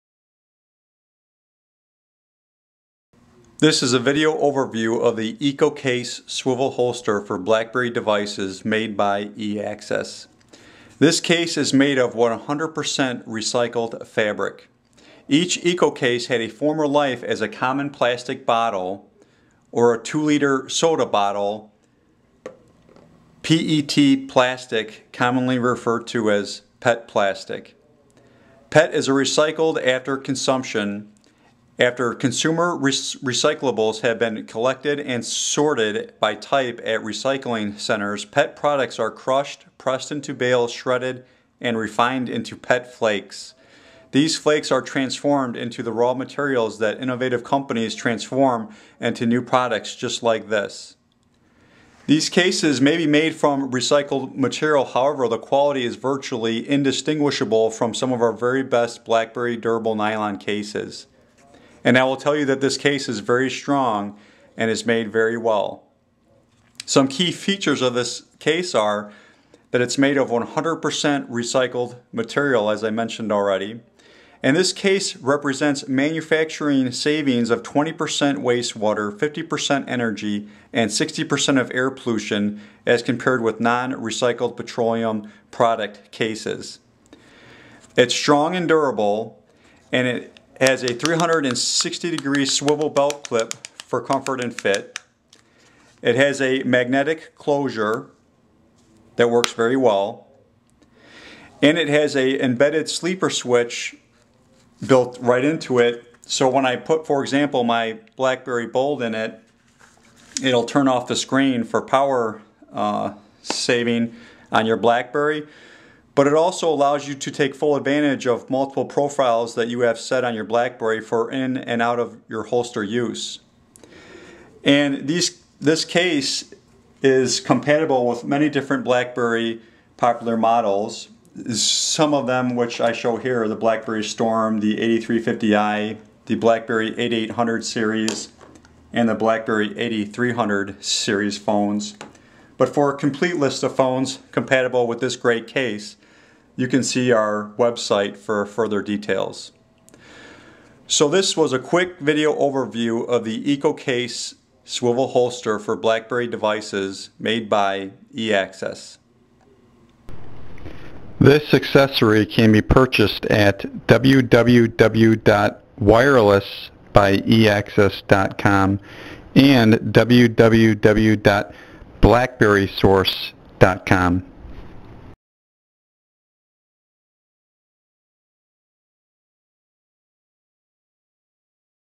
BlackBerry EcoCase Swivel Holster Made from Recycled Material Demo
Made from 100% recycled fabric, this BlackBerry case is durable and makes a statement about yourself and how you feel about your planet. Each EcoCase had a former life as a common plastic water or 2 liter soda bottle (PET plastic). These cases may be made from recycled material however the quality is virtually indistinguishable from some of our very best BlackBerry durable nylon cases. The EcoCase BlackBerry swivel holster features a convenient 24 position durable swivel clip and a magnetic clasp closure.